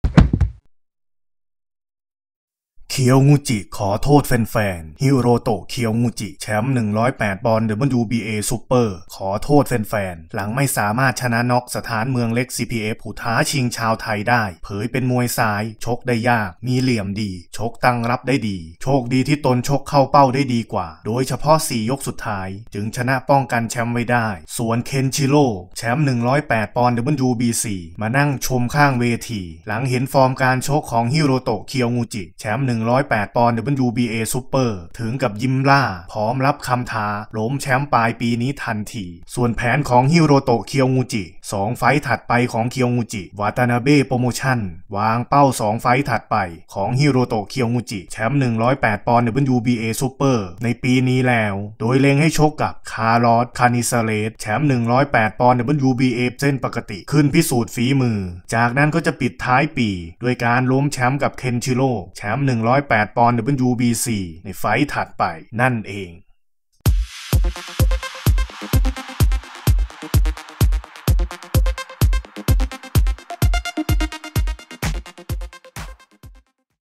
ฮิโรโตะ เคียวงูจิ ขอโทษแฟนๆ หลังน็อค สะท้านเมืองเล็ก ไม่ได้ เตรียมล้มแชมป์ เคน ชิโร่ ปลายปีนี้
Cr NEWS : BBH Fanpage